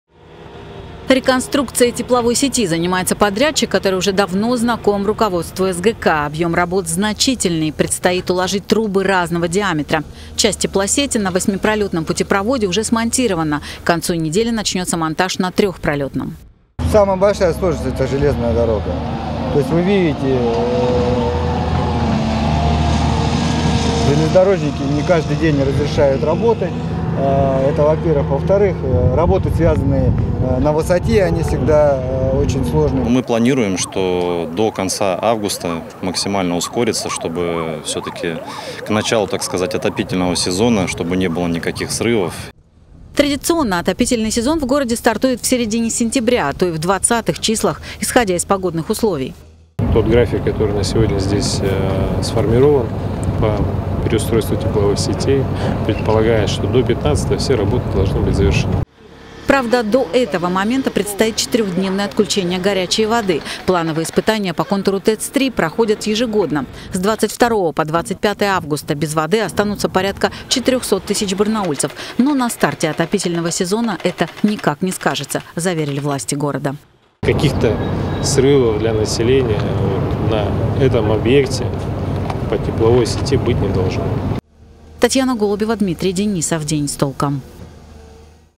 «Максимально ускоримся»: ремонт моста не помешает старту отопительного сезона
Отопительный сезон в Барнауле  начнется вовремя. Об этом накануне сообщили власти города и руководство СГК. Задержек не будет,  несмотря на то ,что в районе строительства нового путепровода  как раз сейчас  прокладывают  теплосети